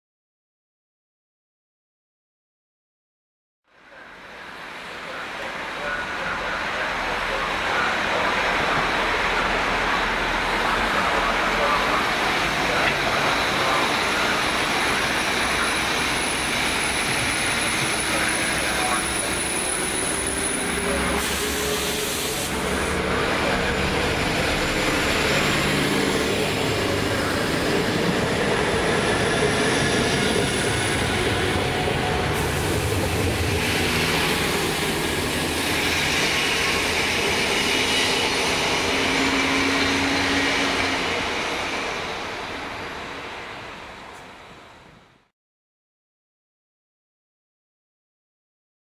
SA132-007 Pociąg osobowy Regio relacji Gdynia Główna - Hel
SA132-007

Odjeżdża z peronu 5 stacji Gdynia Główna jako pociąg osobowy Przewozów Regionalnych nr R90519 relacji Gdynia Główna - Hel.
Na końcu w składzie jedzie SA132-006.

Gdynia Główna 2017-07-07
kolej.krakow.pl